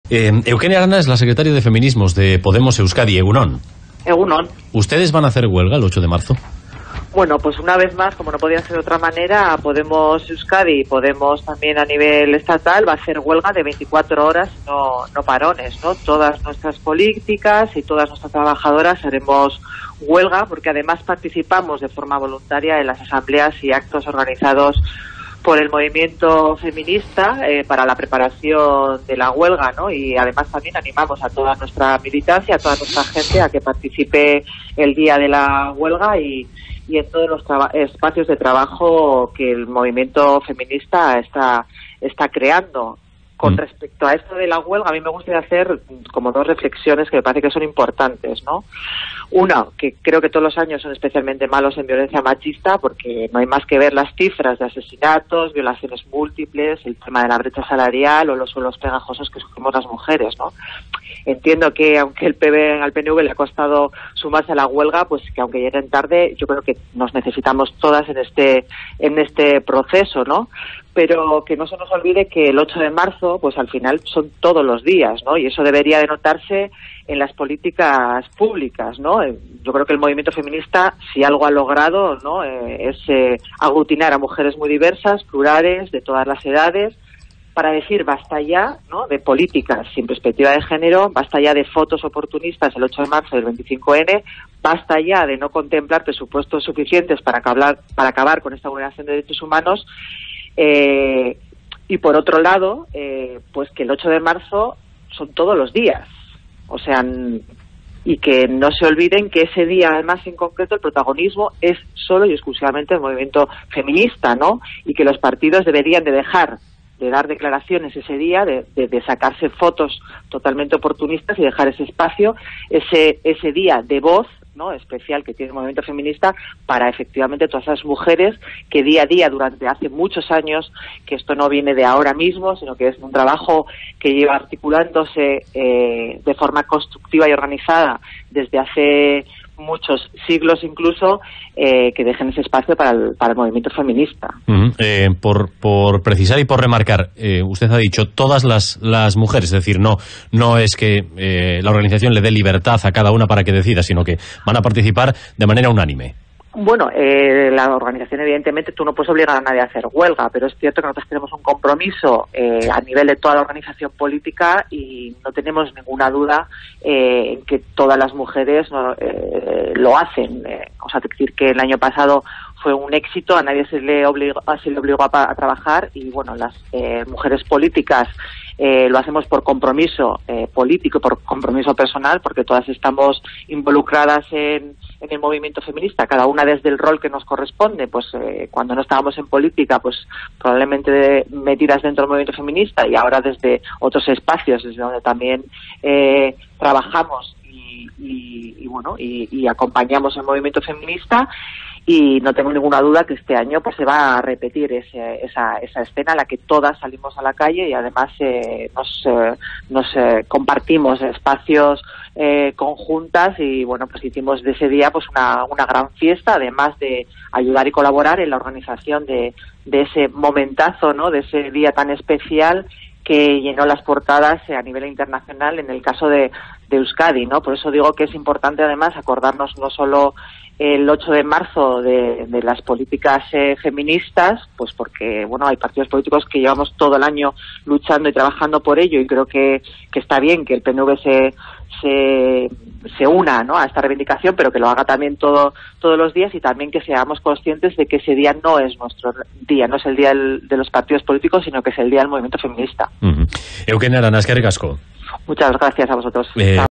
Eukene Arana Secretaria de Feminismos de Podemos Euskadi
Entrevistada en Radio Euskadi sobre la HuelgaFeminista del 8M